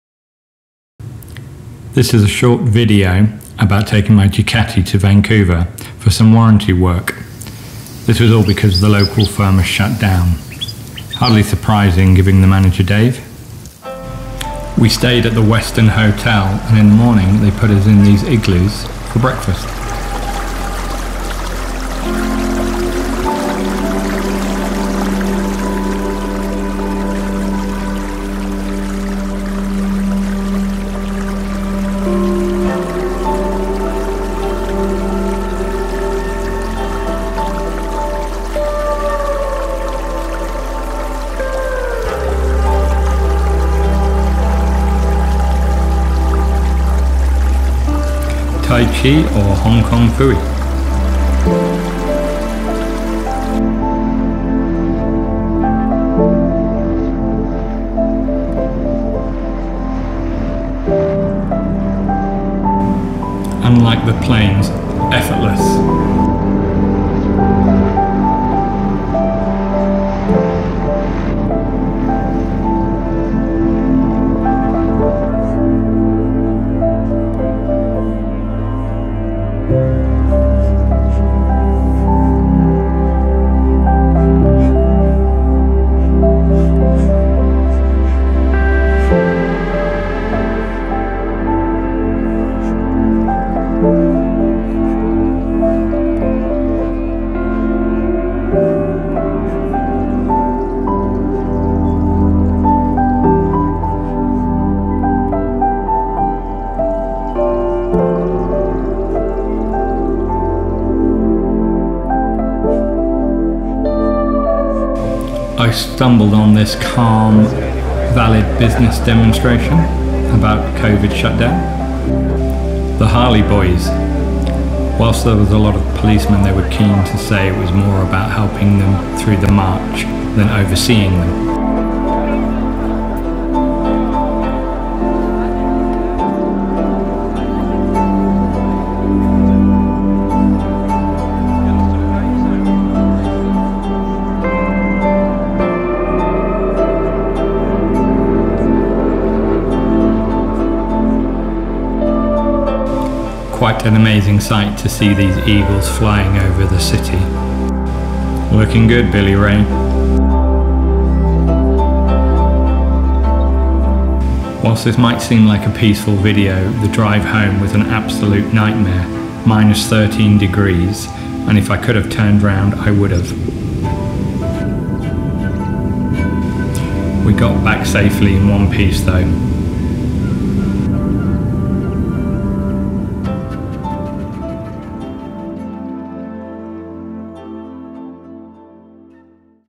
Bike to Vancouver
Short video of us taking my Ducati all the way to Vancouver because the Kelowna dealer rightly went out of business, Dave, what a muppet.  I was going to ride there and back, so glad I listened to local advice..